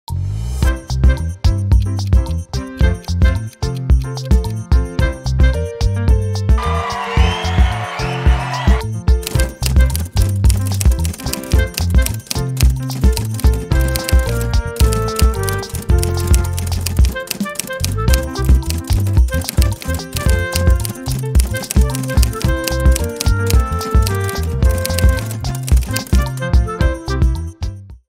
Guess The Animal | Can you guess the dog breed?
Are you a dog lover? Do you think you know all the different dog breeds out there? Join us in this fun game of guessing the dog breed! In this video, we will show you pictures of different dog breeds and challenge you to guess their names. From popular breeds like Golden Retrievers, Chihuahua, Poodle, Bulldog, Beagle, Rottweiler, German Shepherds, Newfoundland to lesser-known breeds like the Xoloitzcuintli and the Pharaoh Hound, we've got them all covered.

Test your knowledge and see if you can identify them all!